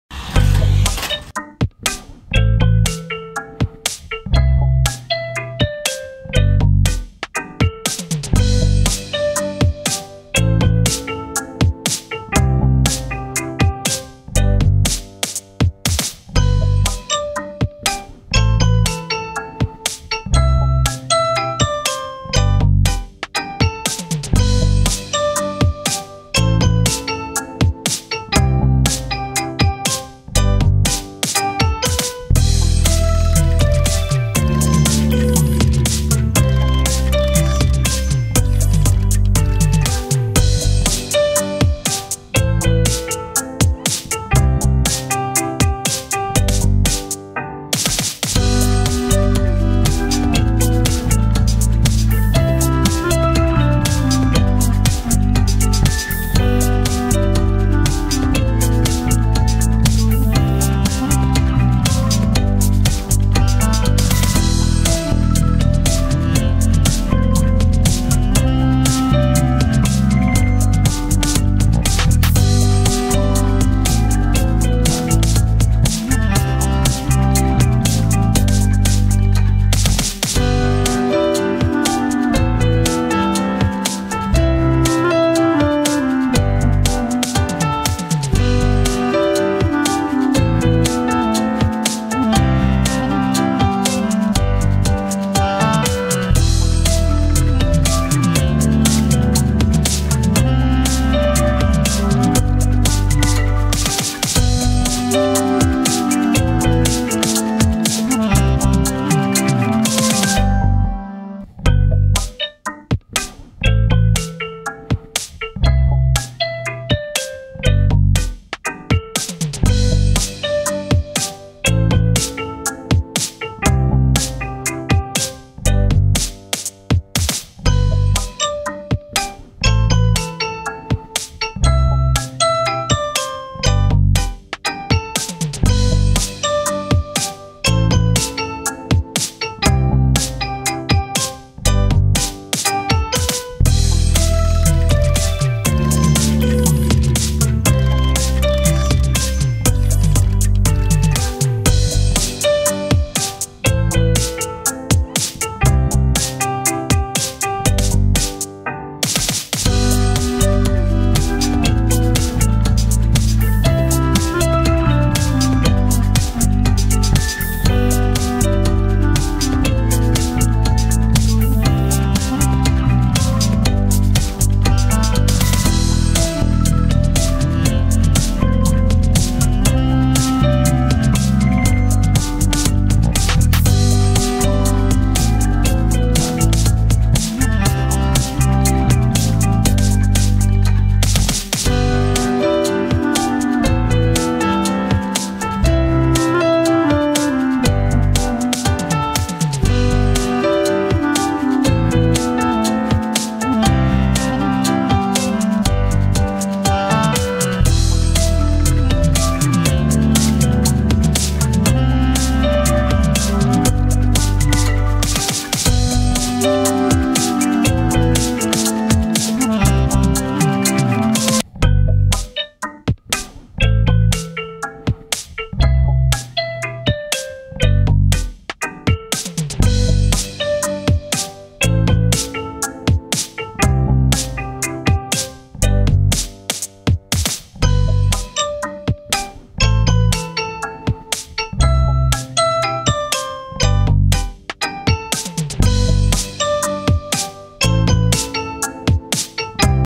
Toys Show RC OFF-Road Bonzer Truck 1:20 Scale | Toy Uboxing | Playing | Stunts
In today's video we are unboxing RC Off Road Bonzer Truck and playing ,doing some stunts and drifts. Enjoy!